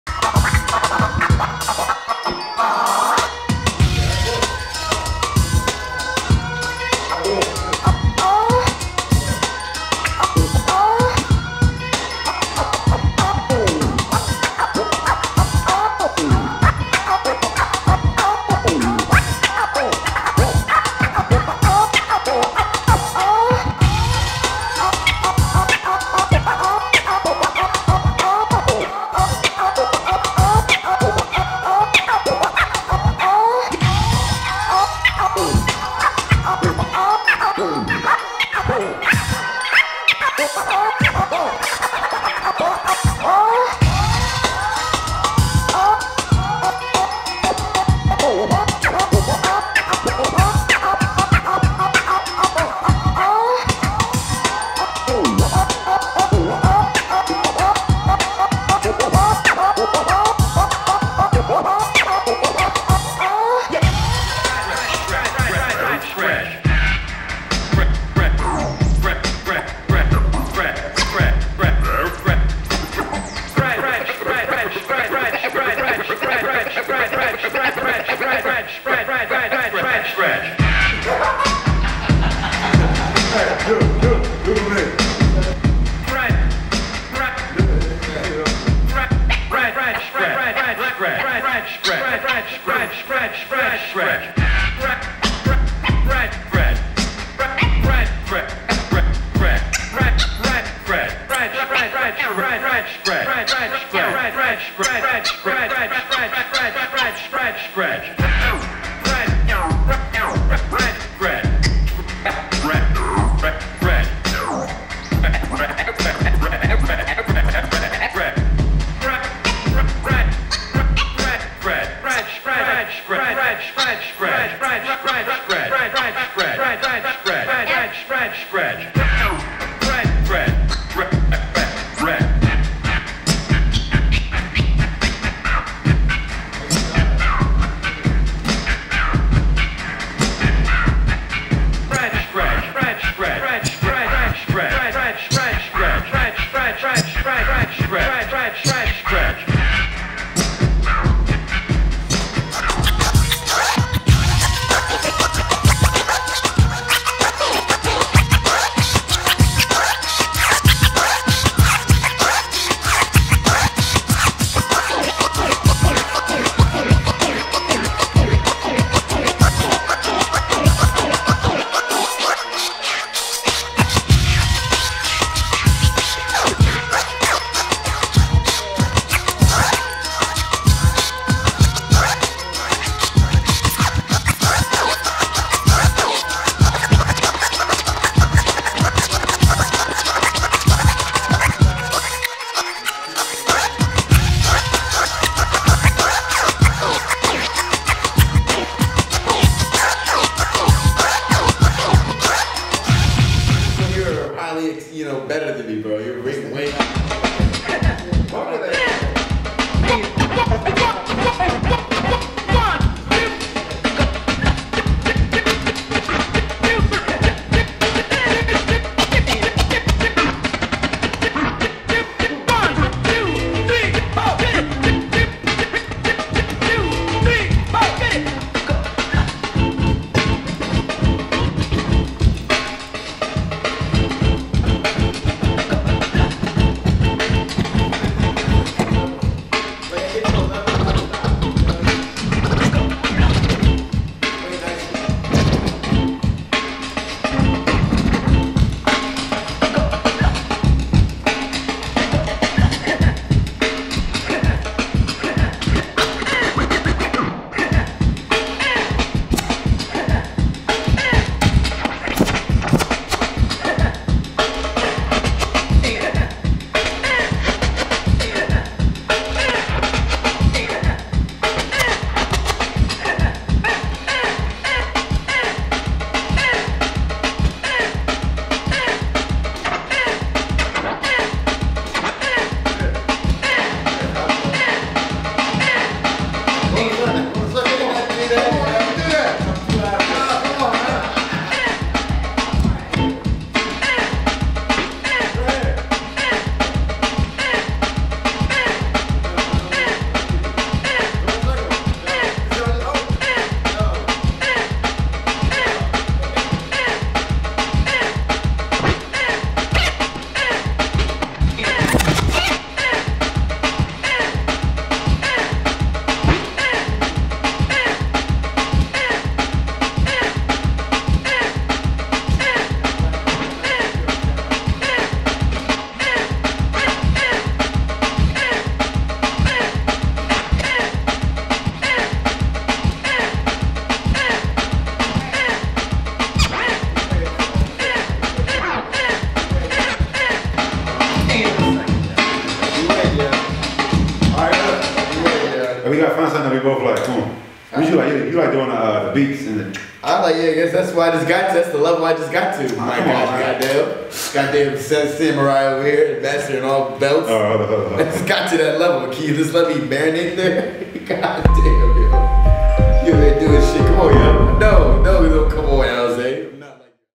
DJ Alizay & DJ Tay James Present: "Silhouettes Series"
Beyond Our Years collaboration with DJ Alizay and DJ Tay James. Live from the Beyond Factory.

Follow the DJs at: @DJTayJames @DjAlizay